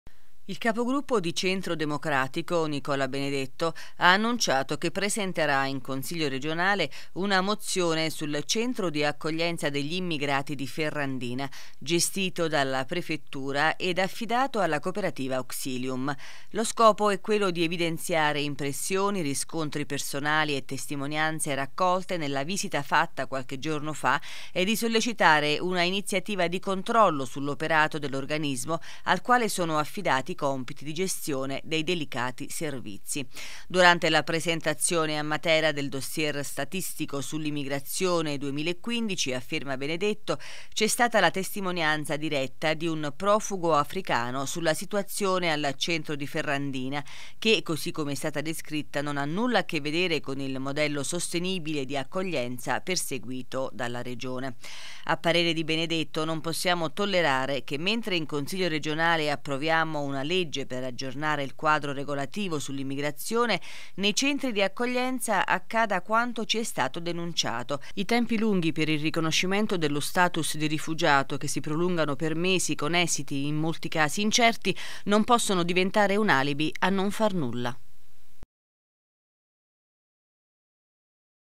CENTRO ACCOGLIENZA FERRANDINA, MOZIONE BENEDETTO
“Necessario controllo su chi gestisce la struttura”